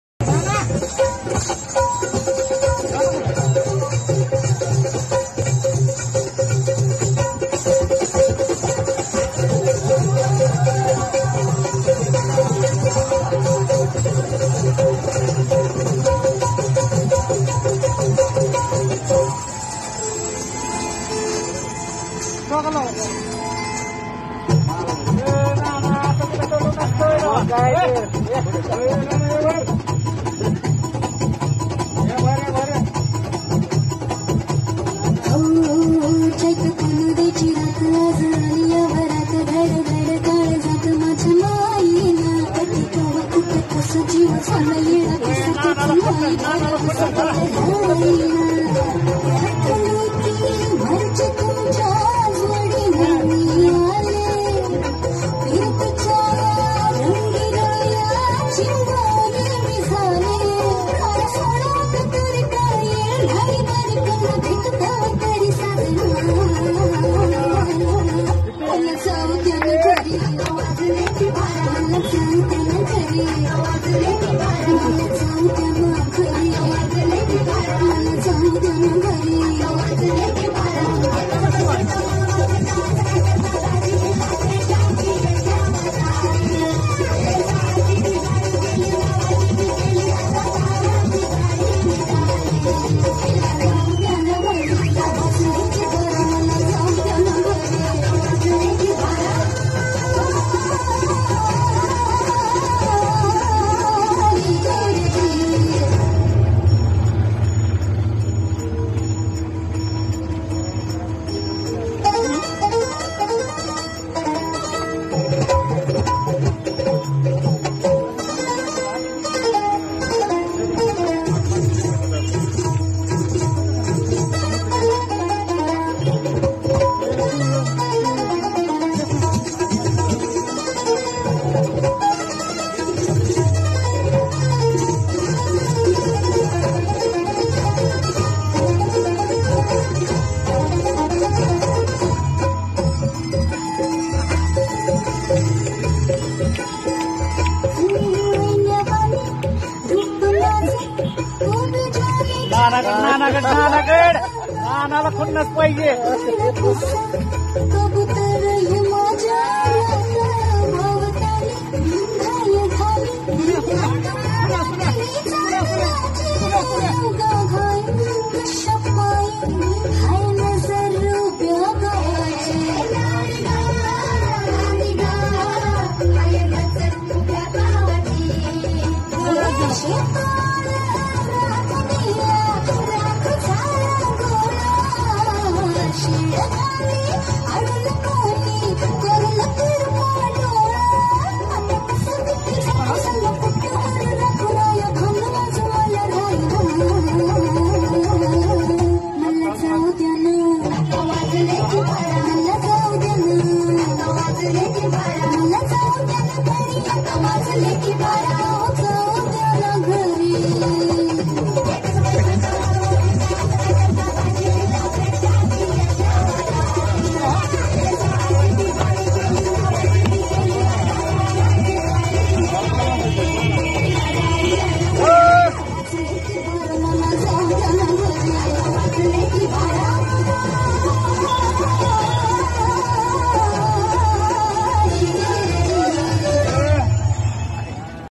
ऑटो ड्राइवर का वायरल डांस
बारामती के ऑटो रिक्शा ड्राइवर बाबाजी कांबले का एक वीडियो सोशल मीडिया पर खूब शेयर किया जा रहा है जिसमें वो कुछ और ऑटो रिक्शा ड्राइवरों के सामने महाराष्ट्र में महिलाओं  द्वारा किया जाने वाला पारंपरिक लोकनृत्य लावणी करते नजर आ रहे हैं. 45 साल के बाबाजी कांबले पिछले कई साल बारामती शहर में ऑटो रिक्शा चला रहे हैं. उनके परिवार में पत्नी और दो बेटे हैं. एक बेटा दसवीं में और दूसरा सातवीं में पढ़ता है।